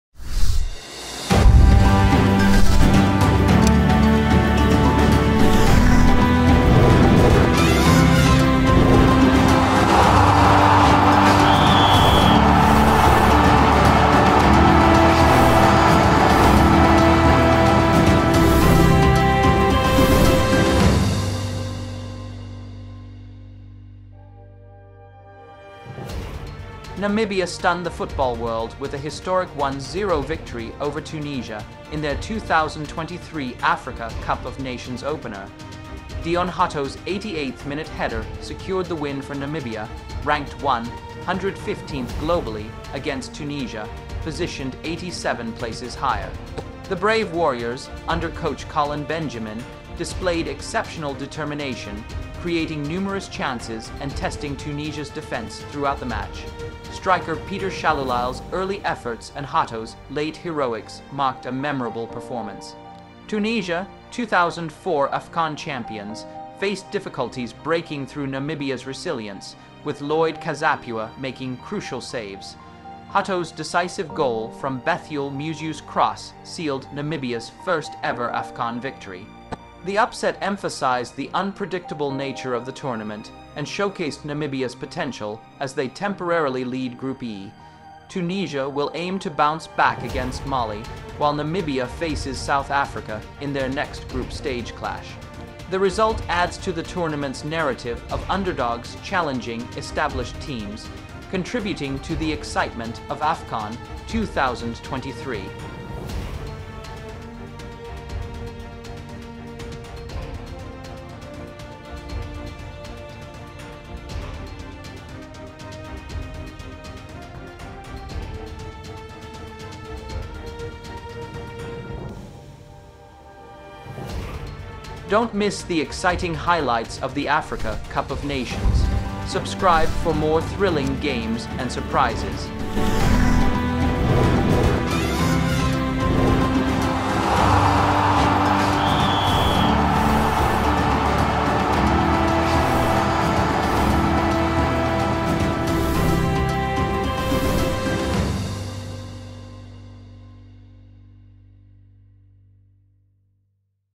🔴 Tunisia vs Namibia | Group E | CAF Africa Cup of Nations | Highlights
Namibia pulled off a stunning upset in their 2023 Africa Cup of Nations opener, defeating Tunisia 1-0 with Deon Hotto's 88th-minute header. The Brave Warriors, ranked 115th globally, secured their first Afcon victory against the 87th-ranked Tunisia. Striker Peter Shalulile's earlier efforts and Namibia's persistent attacks set the tone in a first half where they dominated. Tunisia struggled to break through, with Lloyd Kazapua making crucial saves. Hotto's late goal, from Bethuel Muzeu's cross, sealed the historic win. Namibia's underdog triumph tops Group E temporarily, leaving Tunisia to regroup before facing Mali, while Namibia meets South Africa in their next match.